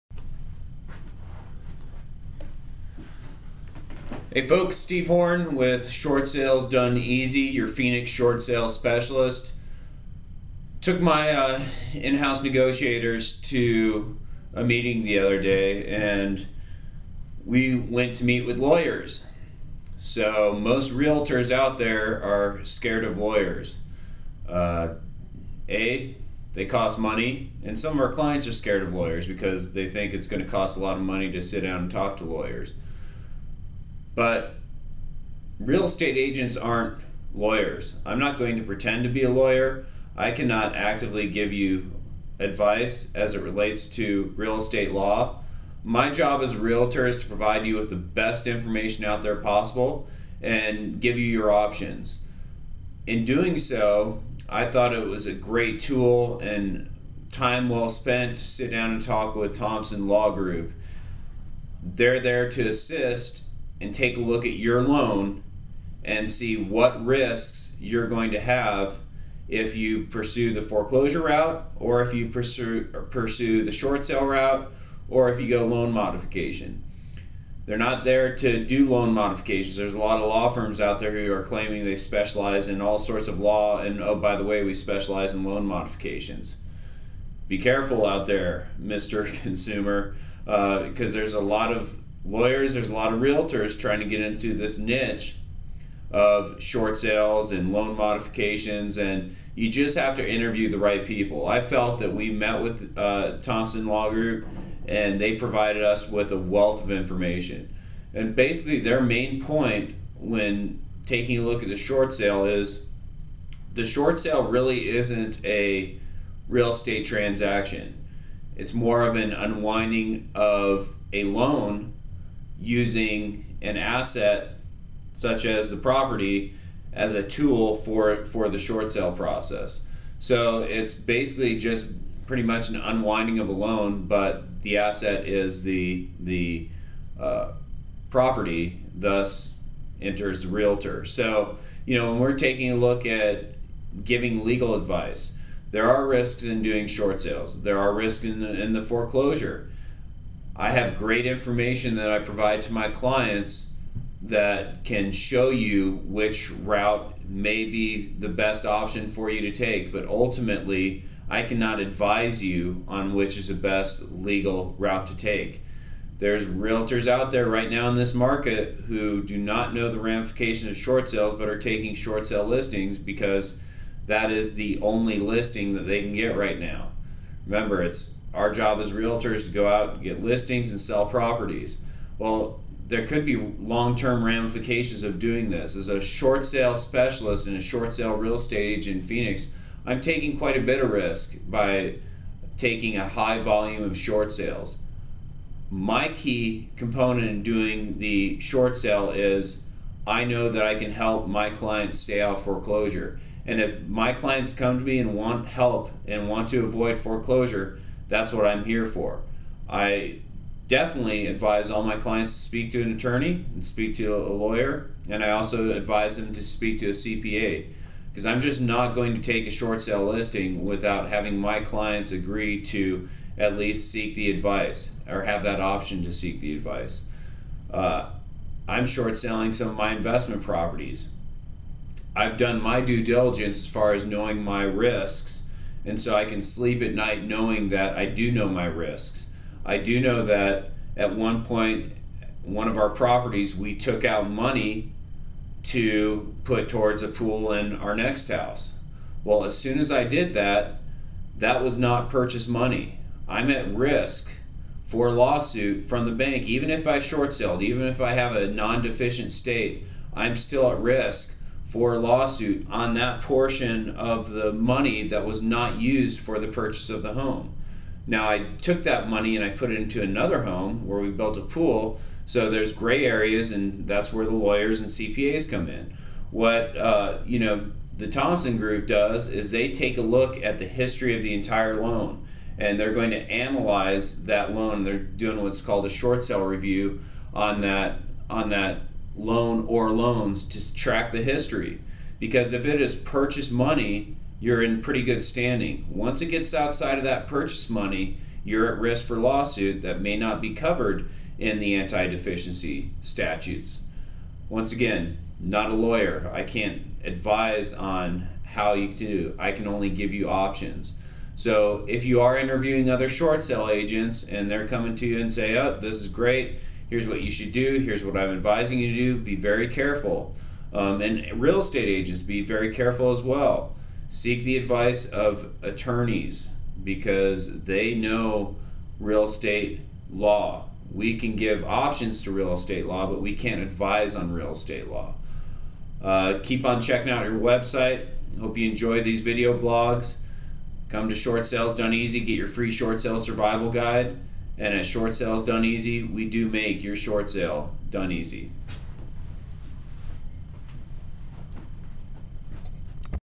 Short Sales And Legal Advice
As a Phoenix Short Sale Specialist, I think that it is vitally important that I continue to educate myself as to the risks involved in short sales.  In order to do this, the Steve Horn Team went and visited the law offices of Thomson Law who specialize in real estate law as it relates to foreclosures and short sales.  This group was a wealth of knowledge and it put into perspective our role as realtors in the short sale process.  As a Phoenix short sale real estate agent I was reminded of several items:

Realtors are not lawyers and we as realtors can not give legal advice when it comes to deficiencies and judgments---this is what lawyers are for.

Our job as a short sale specialist is to price the home correctly, secure the buyer, negotiate on behalf of the seller.

As a Phoenix short sale real estate agent, my job is to find a win win solution between the bank and the seller.

As we listened to the advice, I was reminded by the staff at Thomson Law that the short sale is an unwinding or unraveling of a loan and the real estate is merely the asset that is attached to this loan.  As a Phoenix short sale Specialist, I take risk in listing short sale properties based on the tax and legal implications.  I will advise all of my clients to seek legal and tax advice prior to taking in short sale listings.  At Short Sales Done Easy we make short sales done easy however we advise properly as well.